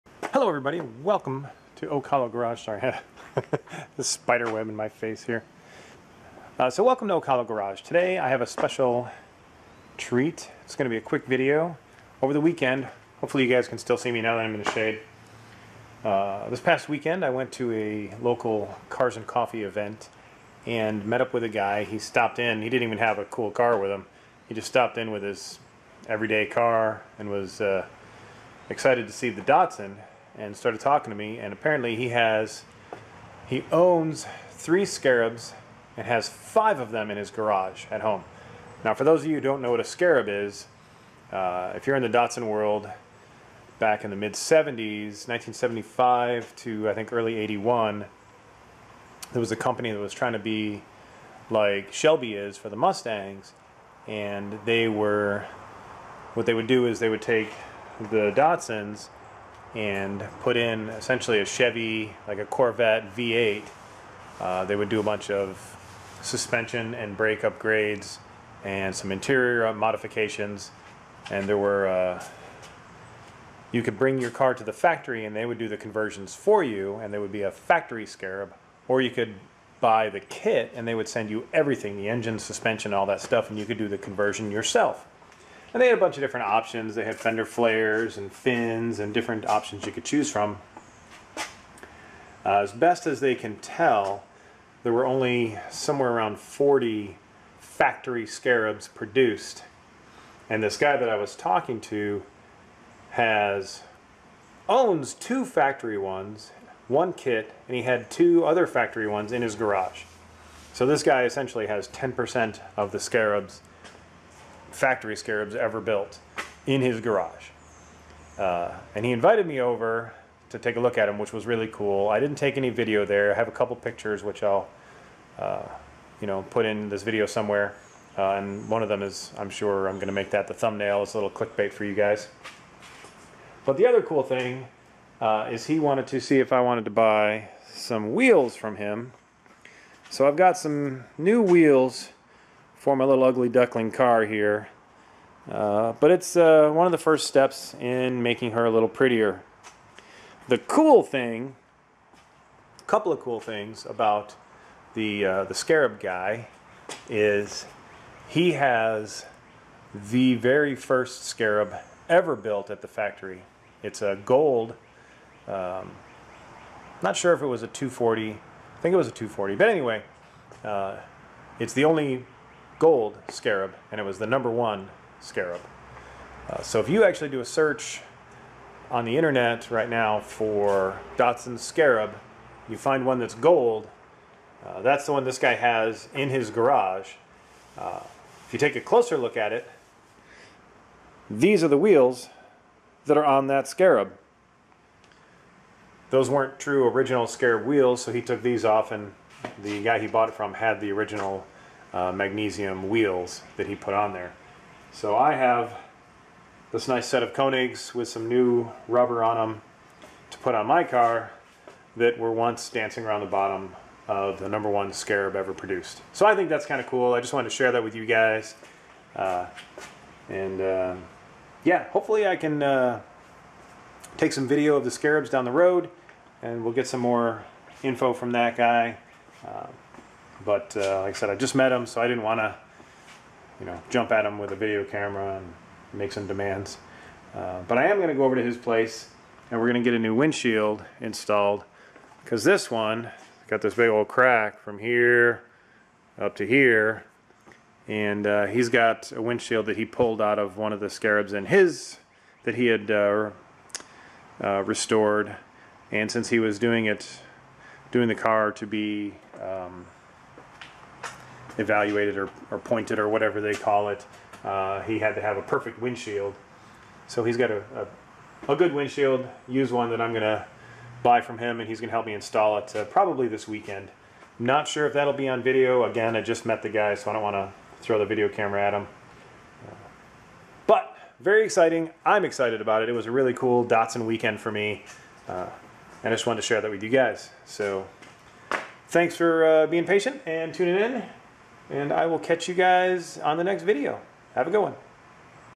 Found A Swarm Of Scarabs And I Have New Wheels To Show For It
I met a guy that owns three Scarabs including the very first Scarab ever produced. He showed me his collection of three Scarabs and two others that he has restored for other people. He also sold me a set of wheels that came on the first scarab when he bought it. All in all, it was a really great Datsun day for me.